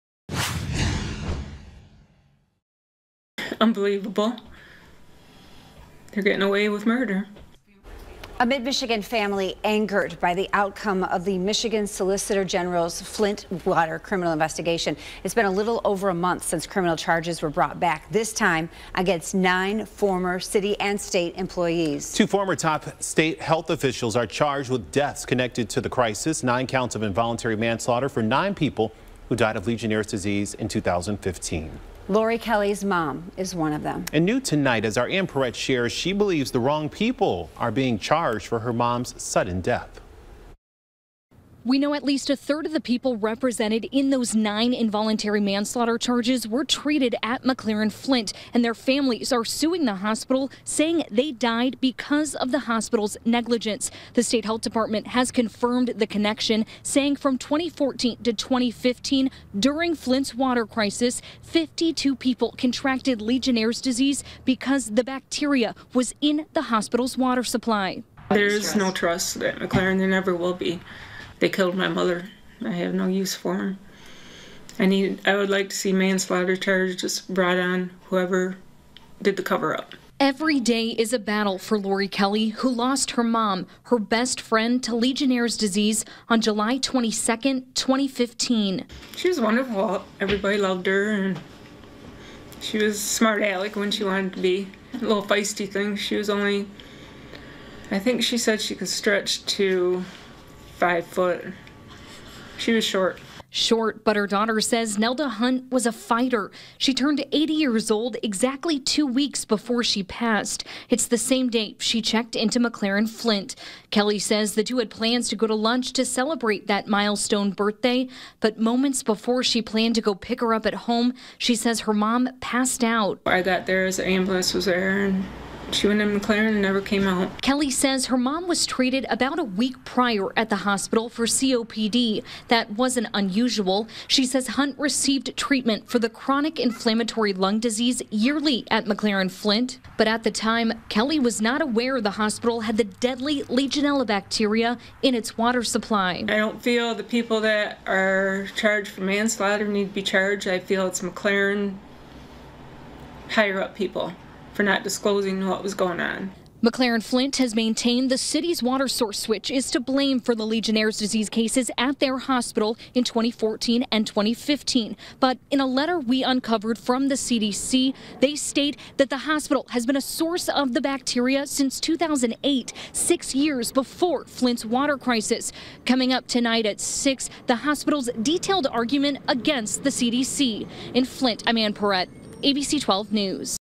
Woman blames McLaren Flint Hospital for her mother's Legionnaires' disease death
Lori Kelly believes the wrong people are charged with her mother’s sudden death.